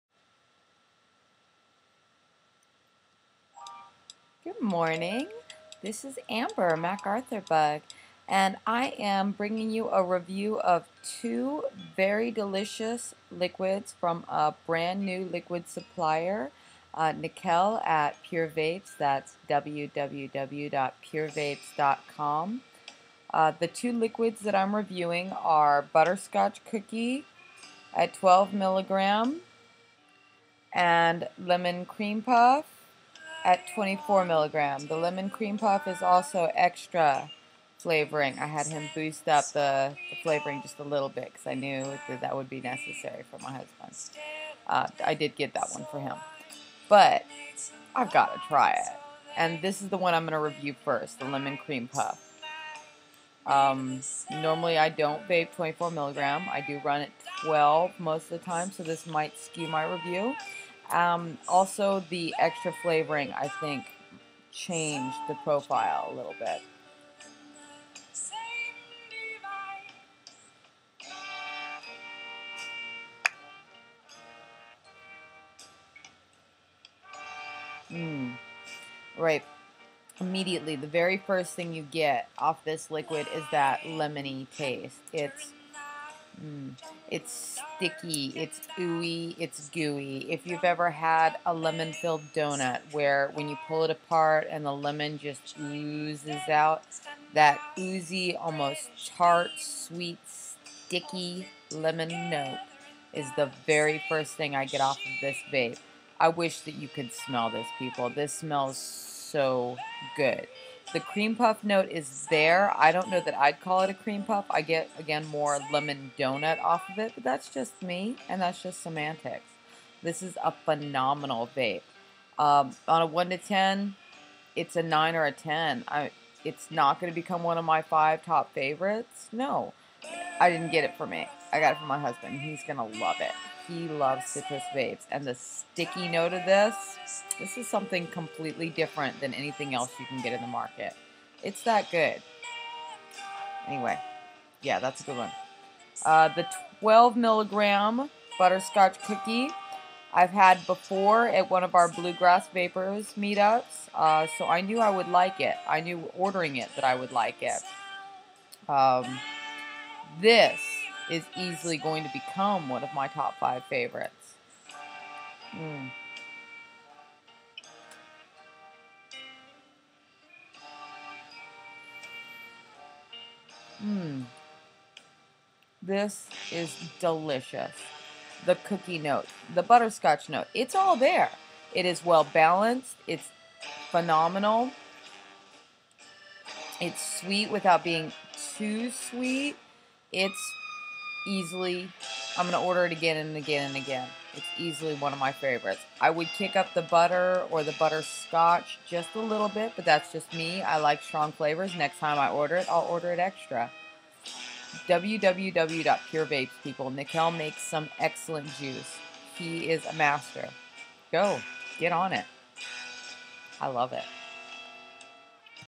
Pure Vapes review
A quickie review of Pure Vapes Lemon Cream puff (24 mg) and Butter Scotch cookie (12 mg)